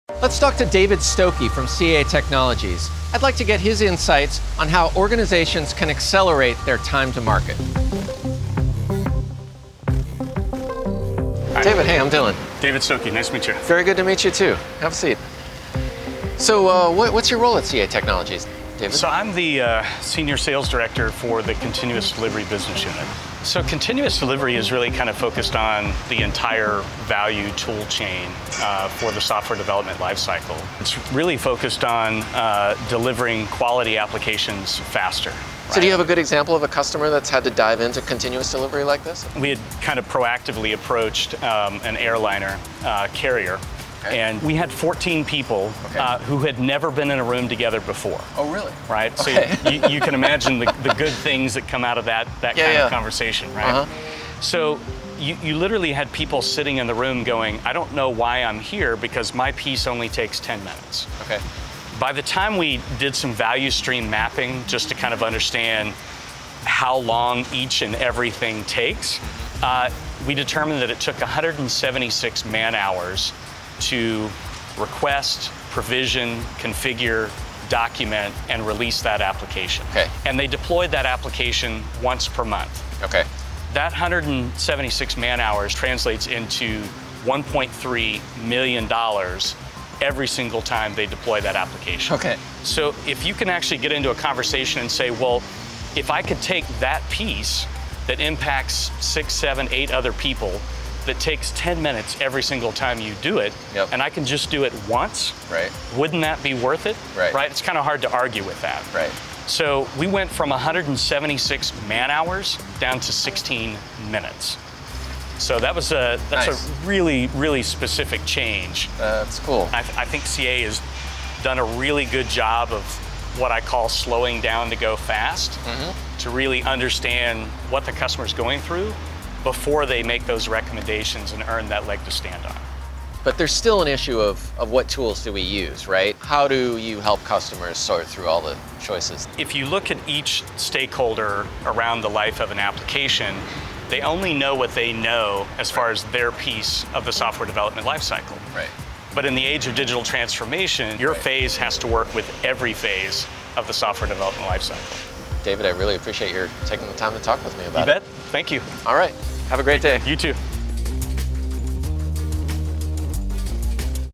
Insider Stories with David Stokey, on Accelerating Time-to-Market with Continuous Delivery
Looking to accelerate time to market for your next app or software release?  David Stokey, Sr. Sales Director at CA Technologies, explains why continuous delivery is a must-have in your software development lifecycle and how he’s worked with customers to reduce their time to market from 176 man hours down to 16 minutes.